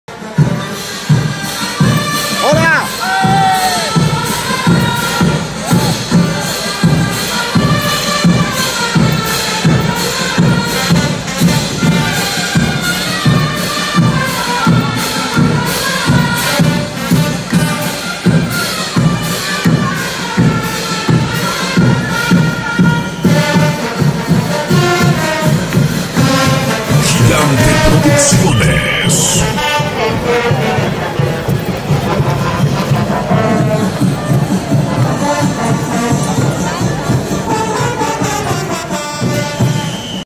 Catalán baila 3 danzas Bolivianas
A Catalán que baila 3 danzas bolivianas lo encontramos bailando CULLAWADA  100x100 Boliviana , en Barcelona - España